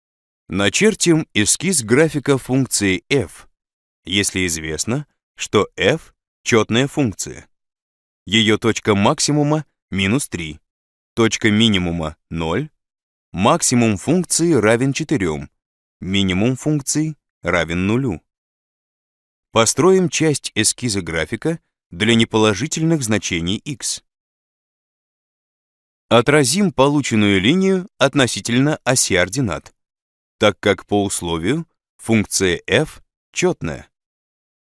Начертить эскиз графика функции f. Задание 3
Начертить эскиз графика функции f, если известно, что f - четная функция, x_max = -3, x_min = 0, f(-3) = 4, f(0) = 0.